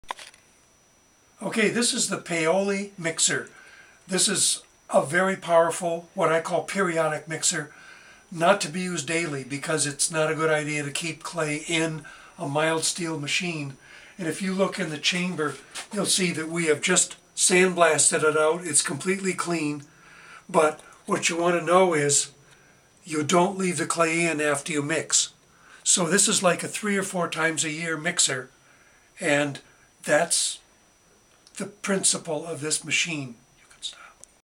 Plum Tree Pottery Clay Mixer/Pug Mill Demo 1
This shows the mixing function (in reverse) and the auger function (in forward motion) when the mixed clay is being pushed out of the machine. Machine built in 1967 by a machinist. Used by me extensively until 1984. Completely cleaned and sandblasted in 2011. Approx. length 7'. Approx. weight 600lbs.

Extremely well built and maintained, someone should take advantage of this wonderful tool.

This is a periodic use mill, owing to the mild steel construction... meaning it is best to use several days to mix large batches of clay and then completely clean and dry until next use.